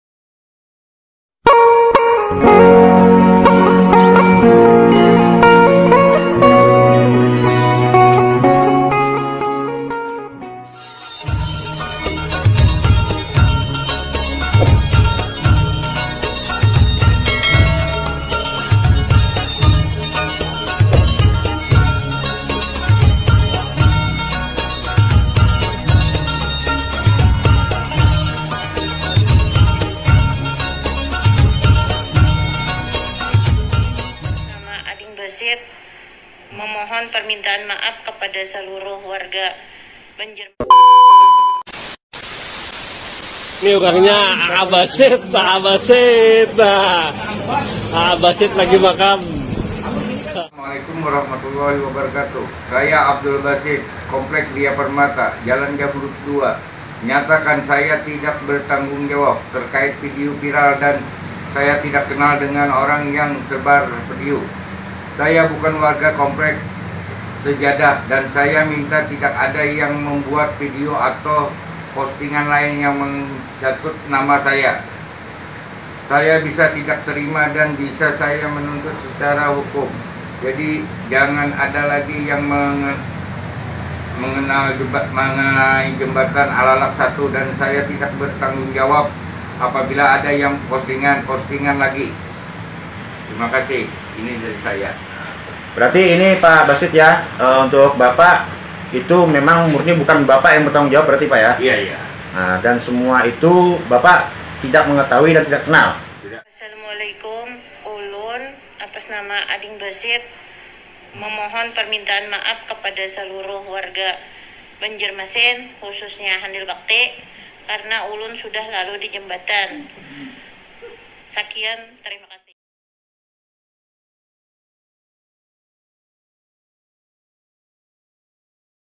video viral 2021||klarifikasi basit jembatan alalak
video viral

VIDEO INI KLARIFIKASI ABANG BASIT YANG LAGI VIRAL DI MEDSOS, TENTANG BEREDARNYA VIDEO BELIAU DAN HAK YANG BERSANGKUTAN